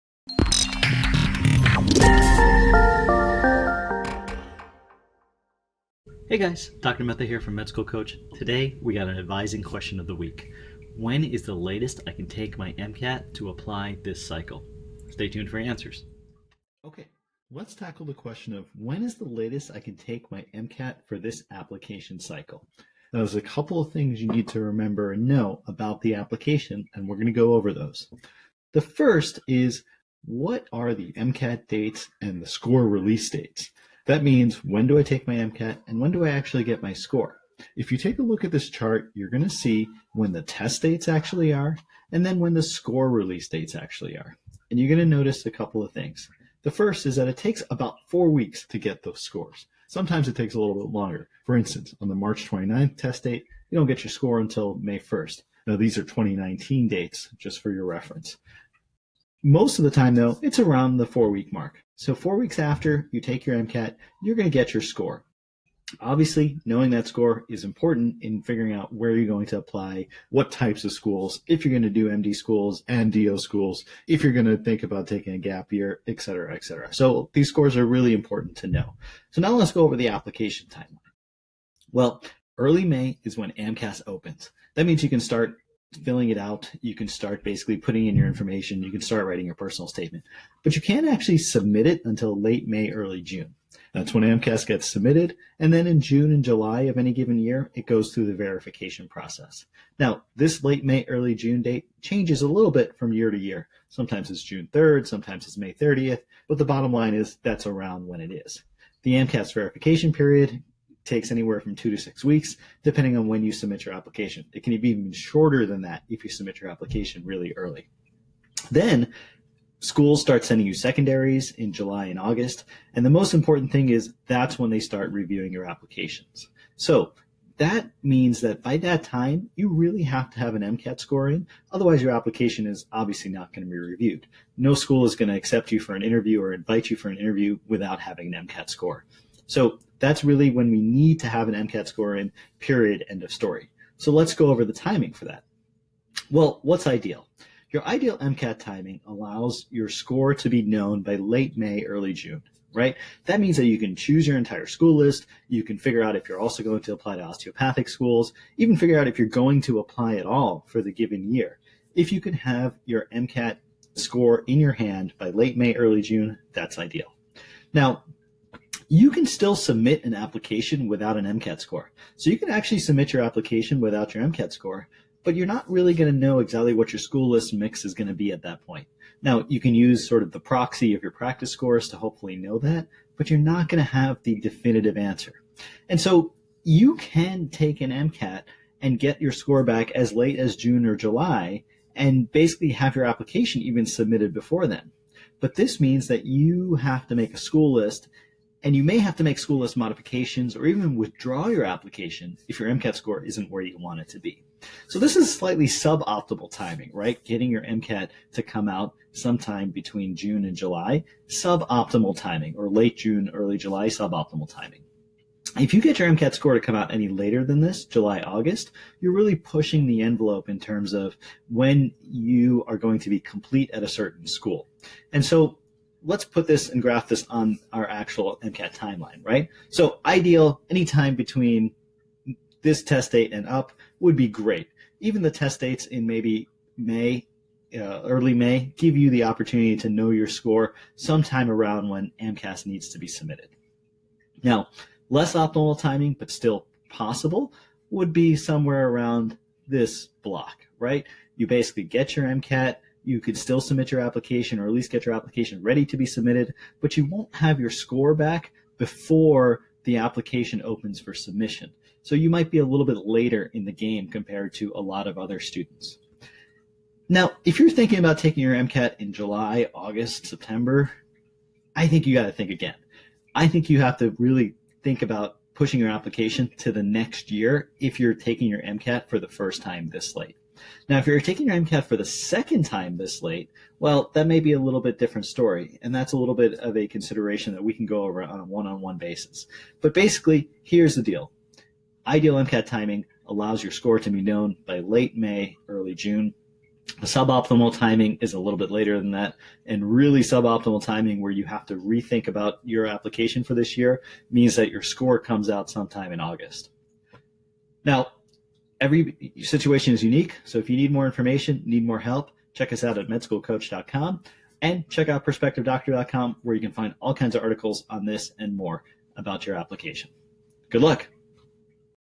When is the Latest I Can Take the MCAT to Apply this Cycle?
If you are applying to medical school this year, you maybe wondering when is the latest you should be taking the MCAT for this application cycle. Dr. Mehta takes you through the answer to this question. Also check out this awesome article to help figure out the best way forward: